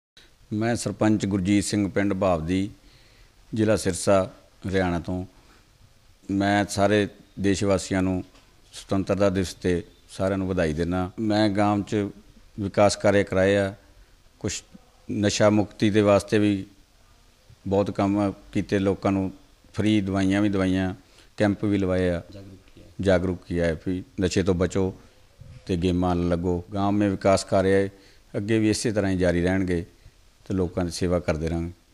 Gram Panchayat Bhawdeen# सभी प्रदेशवासियों को स्वतंत्रता दिवस की हार्दिक बधाई
हरियाणा के सिरसा जिले के गांव भावददीन की ग्राम पंचायत की ओर से सभी देशवासियों को हार्दिक शुभकामनाएं, सरपंच गुरजीत सिंह कंग का ये सन्देश सुनिए, जानिए सरपंच ने गांव में क्या किये विकास कार्य?